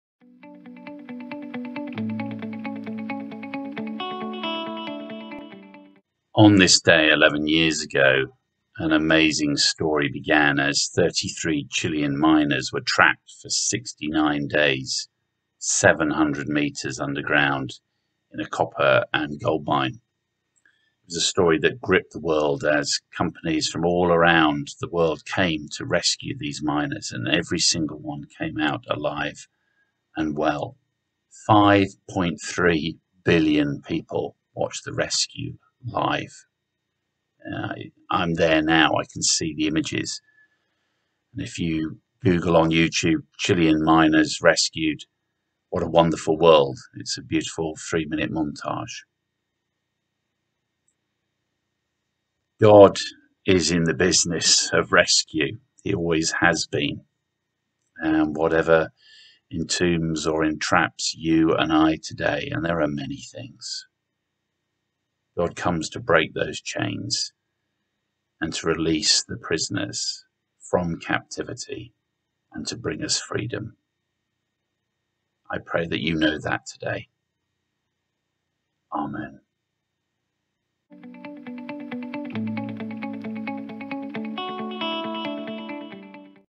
Freedom. A 60 Second Sermon or thereabouts!
Simon looks at the story that gripped the world from 11 years ago today.